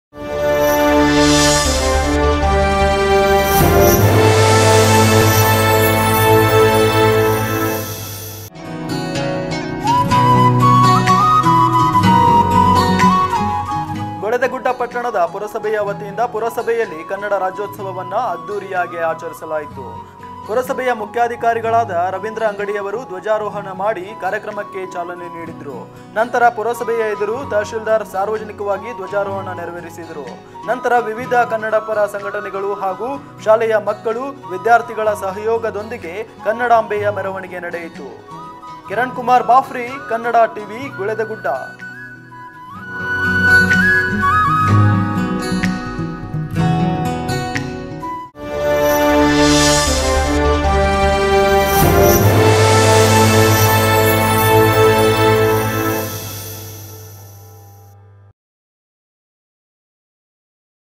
ಗುಳೇದಗುಡ್ಡ ಪುರಸಭೆಯಲ್ಲಿ ರಾಜ್ಯೋತ್ಸವ ಅದ್ದೂರಿ ಆಚರಣೆ
ಪುರಸಭೆಯ ಮುಖ್ಯಾಧಿಕಾರಿ ರವೀಂದ್ರ  ಅಂಗಡಿ ಧ್ವಜಾರೋಹಣ
ವಿಧ್ಯಾರ್ಥಿಗಳ ಸಹಯೋಗದೊಂದಿಗೆ  ಕನ್ನಡಾಂಭೆಯ ಮೆರವಣಿಗೆ